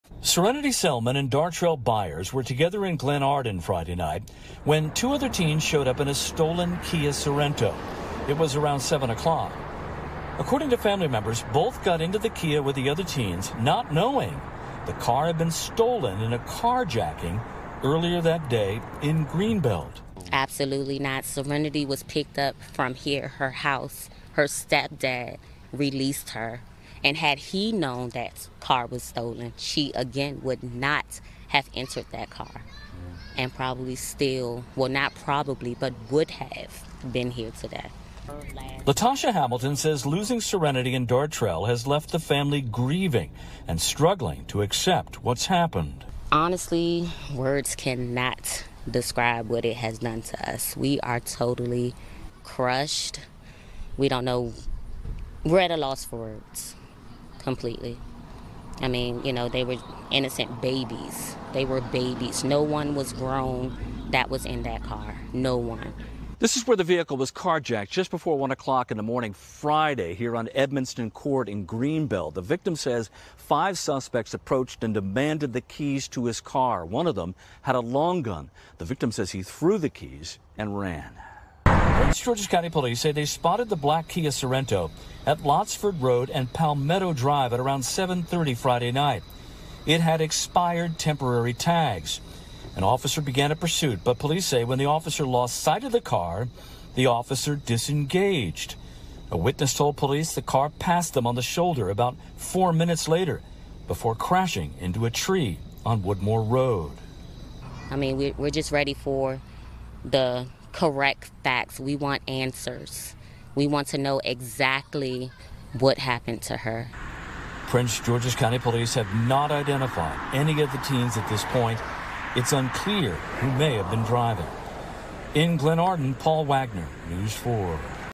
4 teens killed in stolen Kia crash after police pursuit in Prince George's | NBC4 Washington
Family members identified two of the four teenagers who died when a stolen car crashed into a tree in Glenarden, Maryland, Friday evening.